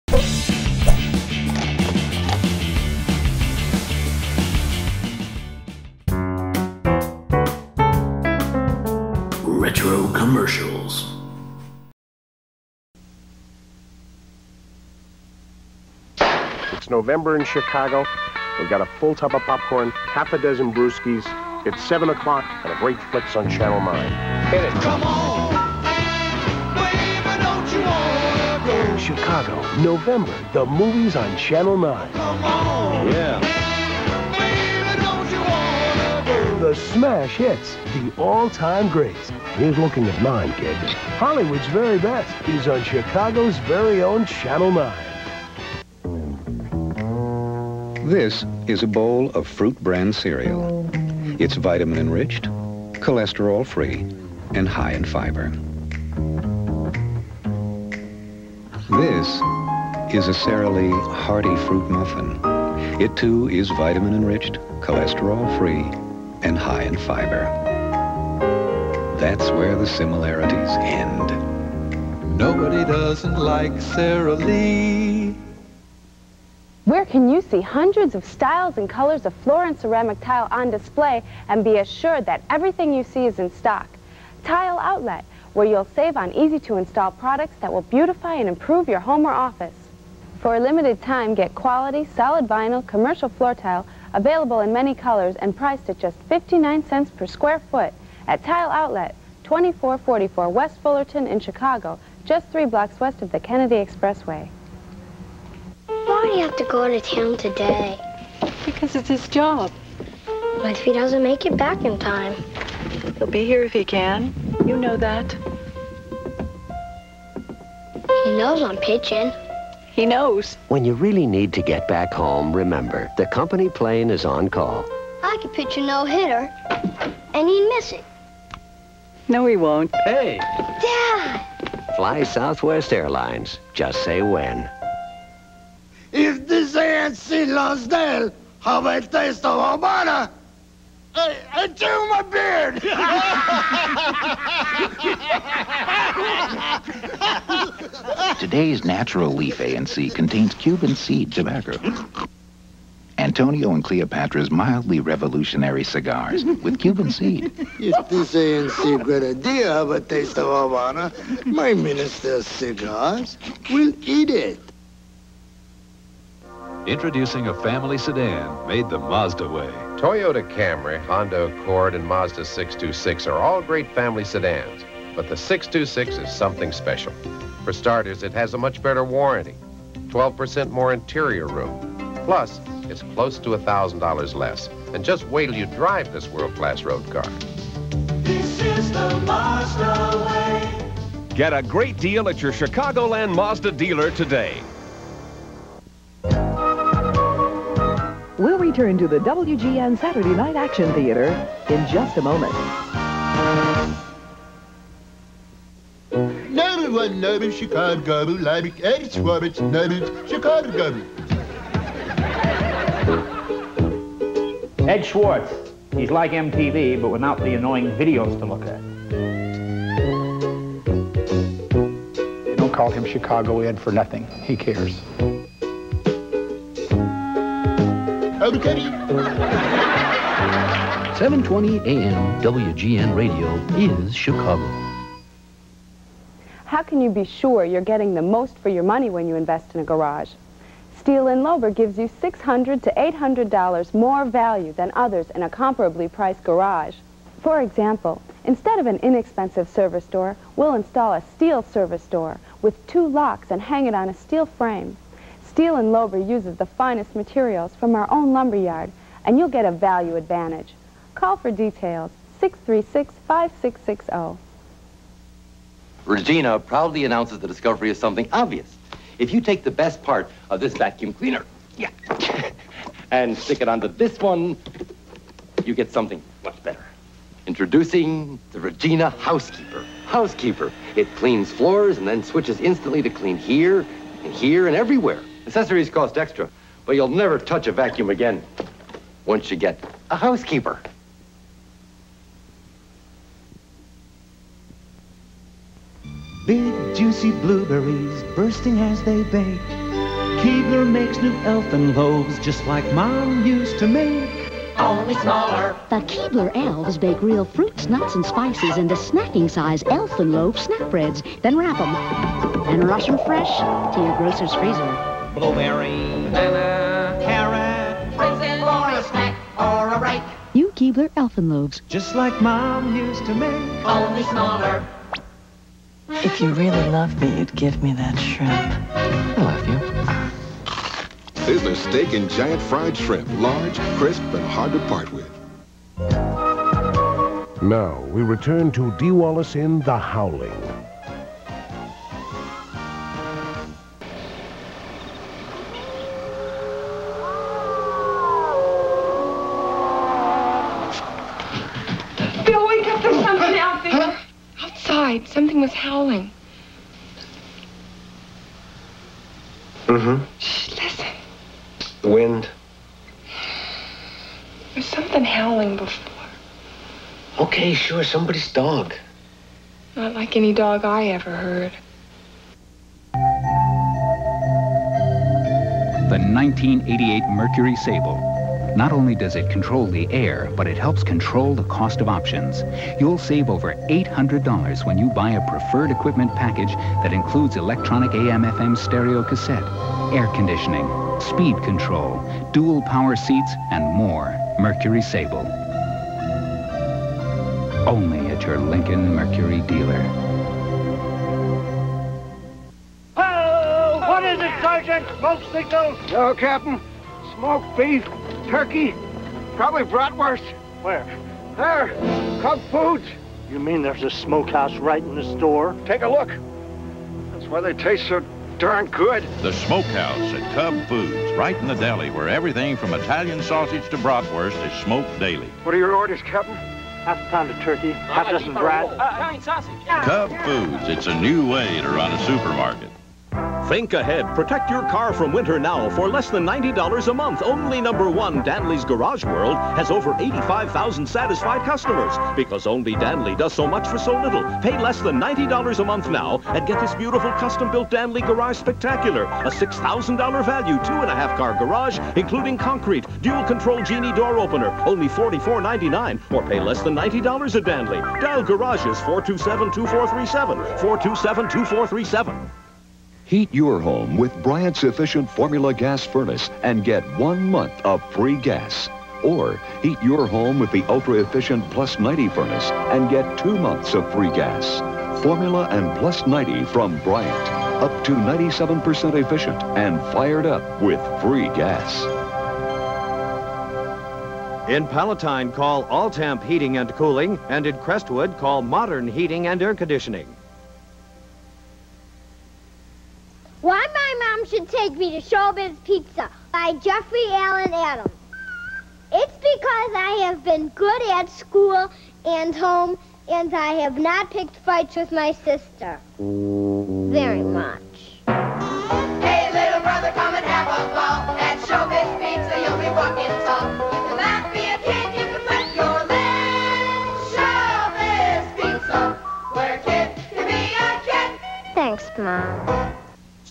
Retro Commercials Vol 256 (1987-HD)
This set of commercials comes from a TV recording of 'The Howling'.  A movie with more animatronics than a Showbiz Pizza performance. Recorded from WGN Channel 9, Chicago. This set has a mix of ads ranging from national commercials to local, hokey ads. Speaking of Showbiz Pizza, you'll see one of the later Showbiz commercials at the end of the episode. Oh, to be a kid again. -Dave

Sara Lee muffins food
Southwest Airlines
Keebler food
Sizzler restaurant
Cub Foods grocery store
Showbiz Pizza